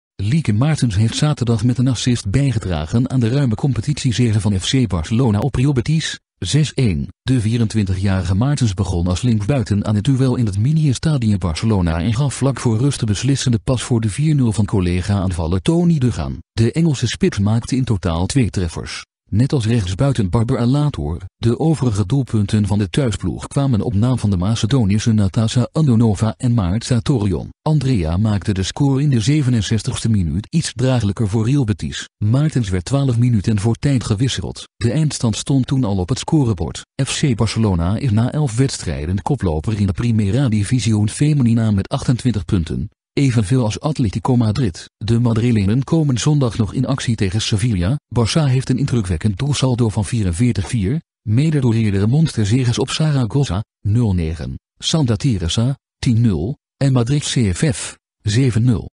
Lieke Martens helt Barcelona weer naar ze overwinning
Lieke Martens heeft zaterdag met een assist bijgedragen aan de ruime competitiezege van FC Barcelona op Real Betis: 6-1.
De 24-jarige Martens begon als linksbuiten aan het duel in het Mini Estadi in Barcelona en gaf vlak voor rust de beslissende pass voor de 4-0 van collega-aanvaller Toni Duggan.

De Engelse spits maakte in totaal twee treffers, net als rechtsbuiten Barbara Latorre. De overige doelpunten van de thuisploeg kwamen op naam van de Macedonische Natasa Andonova en Marta Torrejon. Andrea maakte de score in de 67e minuut iets draaglijker voor Real Betis.

Martens werd twaalf minuten voor tijd gewisseld. De eindstand stond toen al op het scorebord.

FC Barcelona is na elf wedstrijden koploper in de Primera Division Femenina met 28 punten, evenveel als Atletico Madrid. De Madrilenen komen zondag nog in actie tegen Sevilla.

Barça heeft een indrukwekkend doelsaldo van 44-4, mede door eerdere monsterzeges op Zaragoza (0-9), Santa Teresa (10-0) en Madrid CFF (7-0).